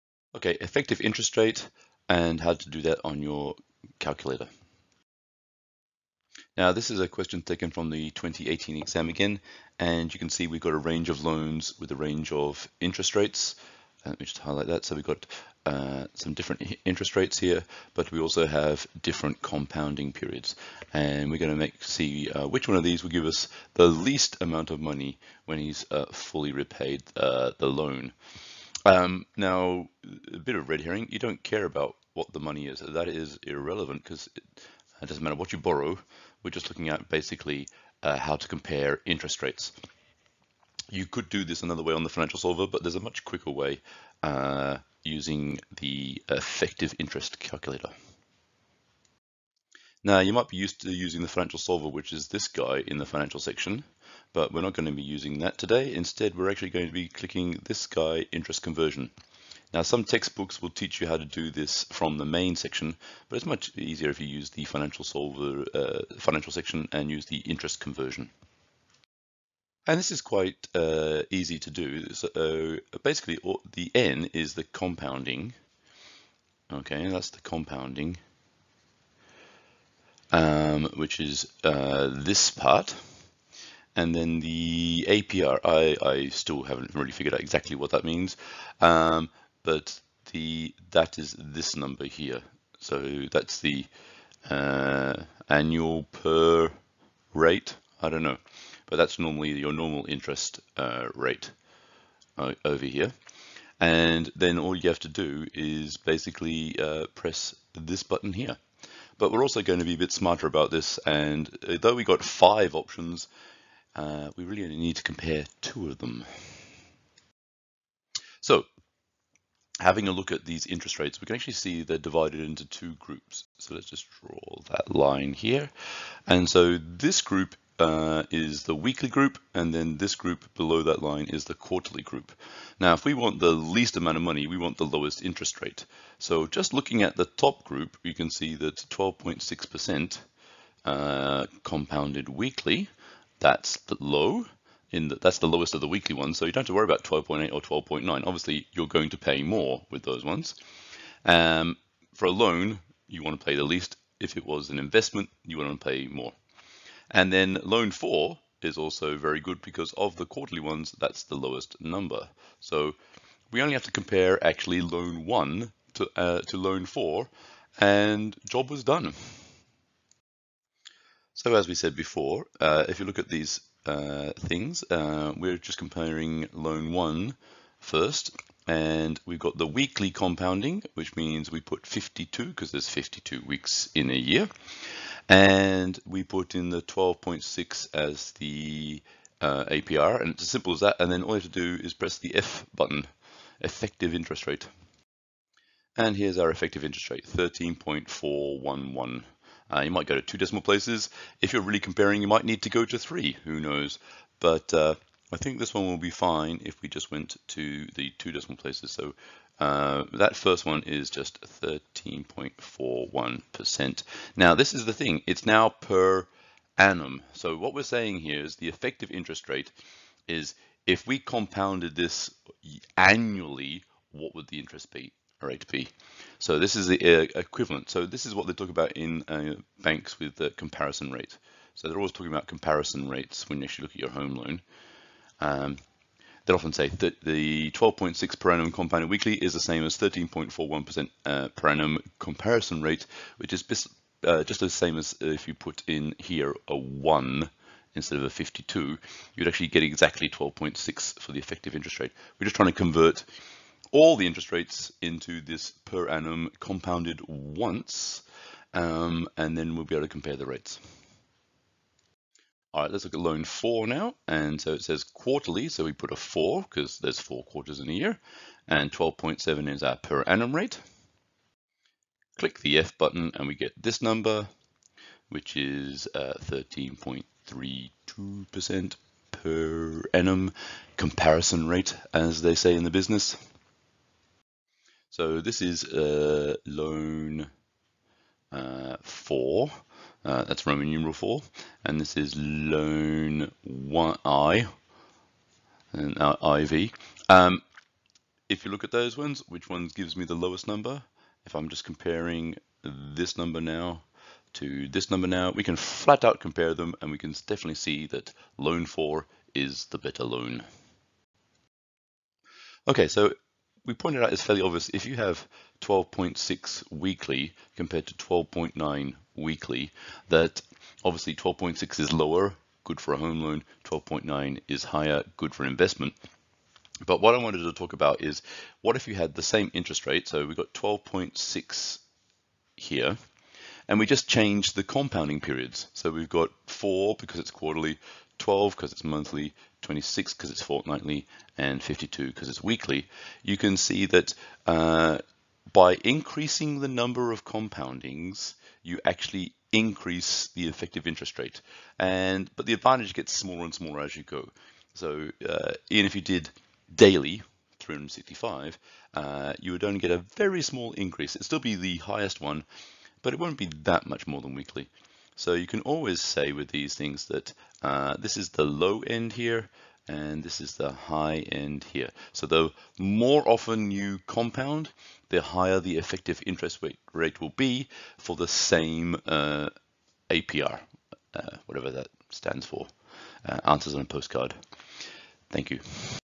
Effective Interest Rate Comparison
How to compare different interest rates using the effective interest rate calculator on your calculator.